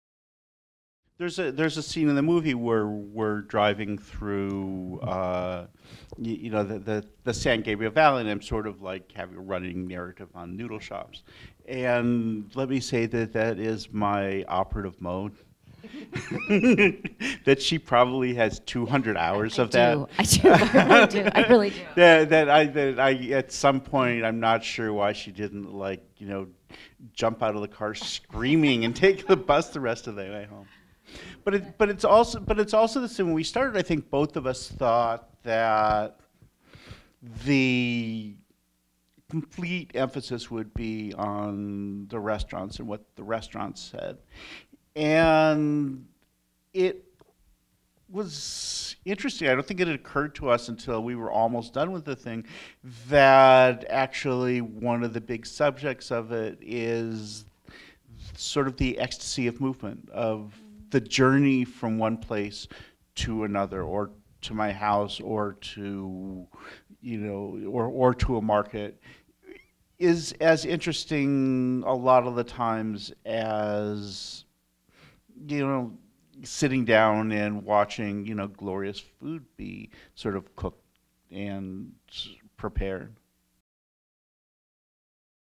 Jonathan Gold on the Focus of City of Gold
Jonathan Gold attended a Q&A at the Jacob Burns Film Center with filmmaker Laura Gabbert and JBFC Programmer Gina Duncan following a screening of City of Gold on February 29, 2016.